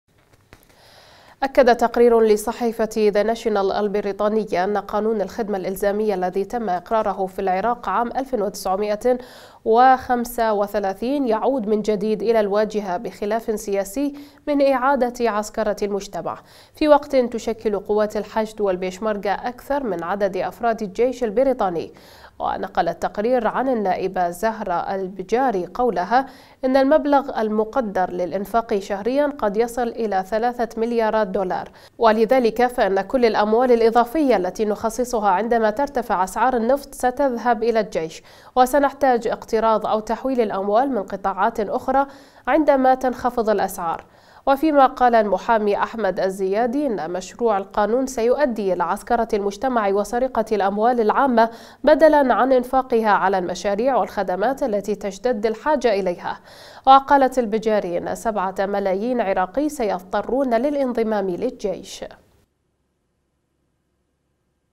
صحيفة بريطانية: قانون الخدمة الإلزامية يعيد العراق الى عسكرة المجتمع
اكد تقرير لصحيفة ذي ناشيونال البريطانية ان قانون الخدمة الالزامية الذي تم اقراره في العراق عام ألف وتسعمئة وخمسة وثلاثين يعود من جديد الى الواجهة بخلاف سياسي من اعادة عسكرة المجتمع، في وقت تشكل قوات الحشد و البيشمركة اكثر من عدد افراد الجيش البريطاني، ونقل التقرير عن النائبة زهرة البجاري قولها: إن المبلغ المقدر للانفاق شهريا قد يصل الى ثلاثة مليارات دولار، ولذلك فان كل الاموال الاضافية التي نخصصها عن دما ترتفع اسعار النفط ستذهب الى الجيش وسنحتاج اقتراض او تحويل الاموال من قطاعات اخرى عند ما تنخفض الاسعار، وفيما قال المحامي احمد الزيادي ان مشروع القانون سيؤدي الى عسكرة المجتمع وسرقة الاموال العامة بدلا عن انفاقها على المشاريع والخدمات التي تشتد الحاجة اليها، وقالت البجاري إن سبعة ملايين عراقي سيضطرون للانضمام للجيش.